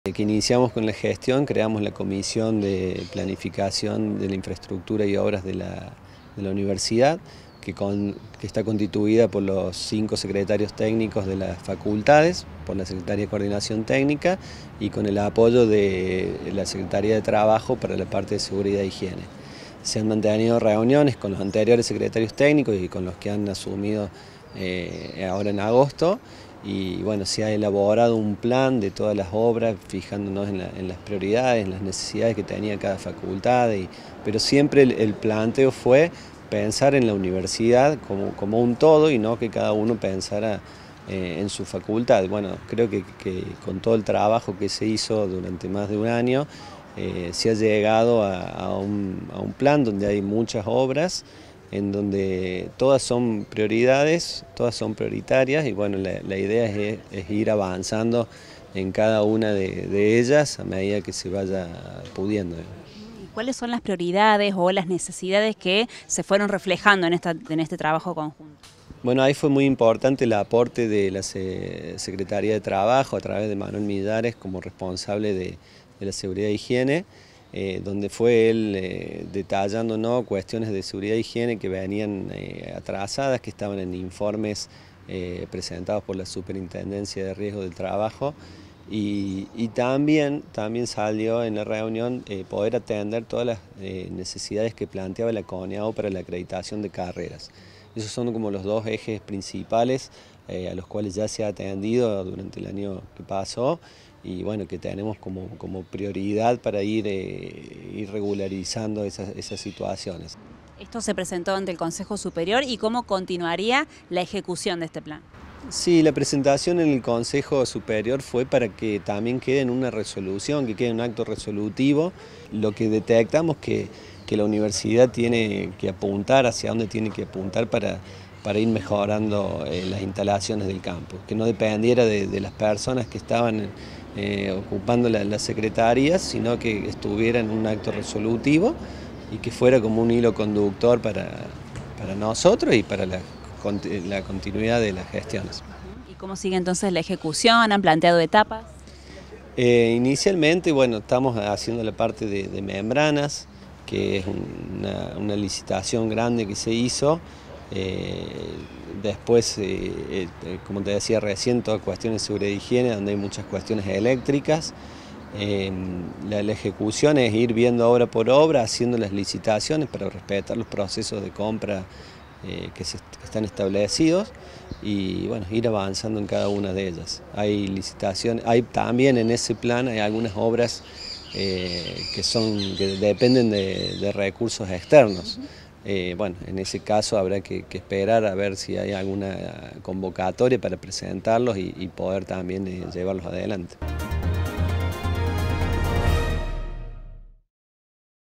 PRESENTARON EL PLAN INTEGRAL DE OBRAS PARA LA UNRC: PRIORIZARÁN MEJORAS EN HIGIENE Y SEGURIDAD
EZEQUIEL TARDIVO – SECRETARIO DE COORDINACIÓN TÉCNICA Y SERVICIOS
UniRio TV
Universidad Nacional de Río Cuarto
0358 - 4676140